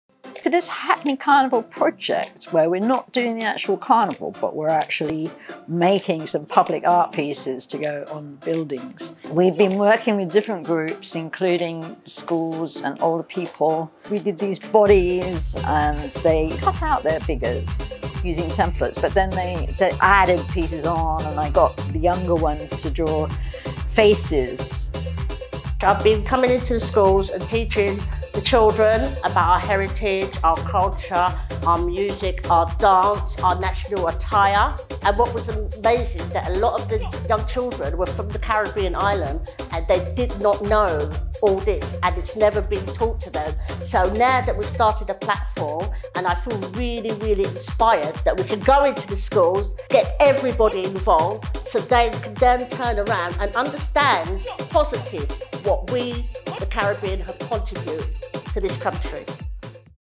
Hackney Carnival kids' workshops II
In 2021, Hackney Carnival had to be different. Due to the pandemic, events and activities took place over the summer including a series of Carnival-inspired artwork workshops in seven schools and three elders groups.

Check out some of the excellent outcomes of those workshops in our video.

For more information on future Hackney Carnival activity